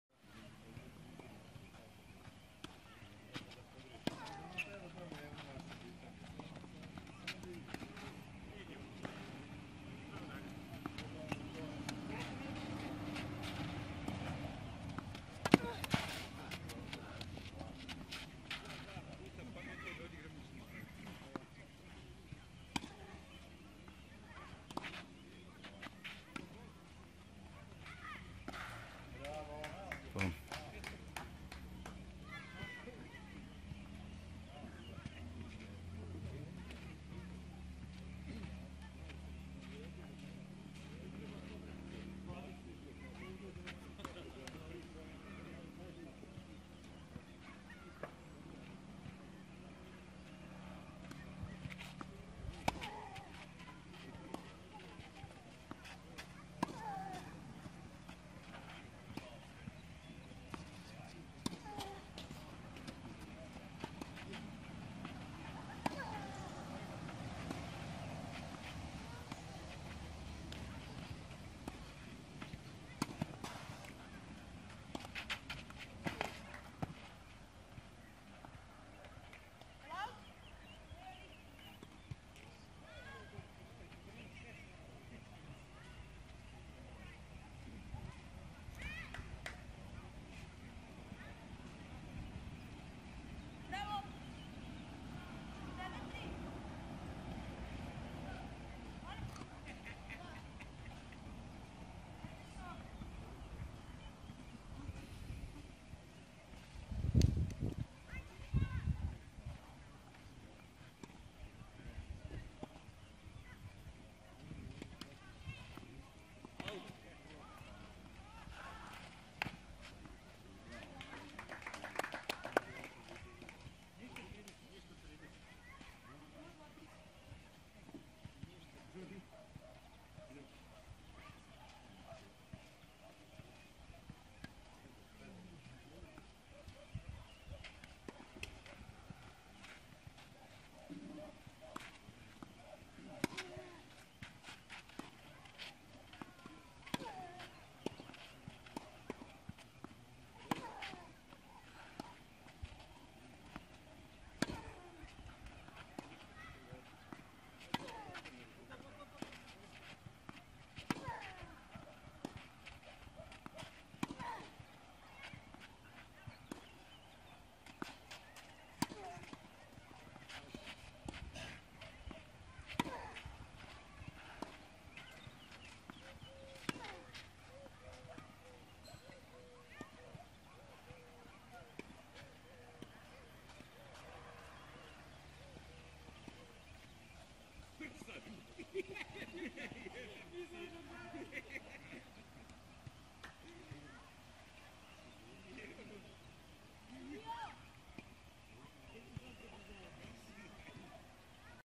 Andrej Rajak - Petar Ćertić, polufinale OP Kikinde do 12 godina, TK Kikinda, 21.5.2017.
Andrej Rajak - Petar Ćertić, polufinalni meč OP Kikinde do 12 godina, odigran u TK Kikinda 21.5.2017.
Andrej je pobedio 6-3 6-3